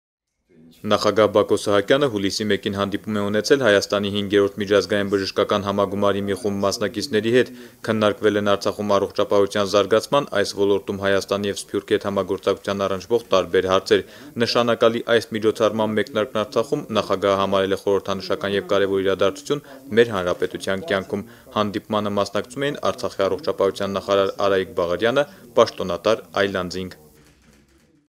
Բակո Սահակյանն ընդունել է Հայաստանի 5-րդ միջազգային բժշկական համագումարի մի խումբ մասնակիցների
Արցախի Հանրապետության Նախագահ Բակո Սահակյանը հուլիսի 1-ին հանդիպում է ունեցել Հայաստանի 5-րդ միջազգային բժշկական համագումարի մի խումբ մասնակիցների հետ:
Ինչպես հայտնել են Արցախի Հանրապետության նախագահի աշխատակազմի տեղեկատվության գլխավոր վարչությունից, քննարկվել են Արցախում առողջապահության զարգացման, այս ոլորտում Հայաստանի եւ Սփյուռքի հետ համագործակցությանն առնչվող տարբեր հարցեր:
Նշանակալի այս միջոցառման մեկնարկն Արցախում Նախագահը համարել է խորհրդանշական եւ կարեւոր իրադարձություն մեր հանրապետության կյանքում:
Հանդիպմանը մասնակցում էին ԱՀ առողջապահության նախարար Արայիկ Բաղրյանը, պաշտոնատար այլ անձինք: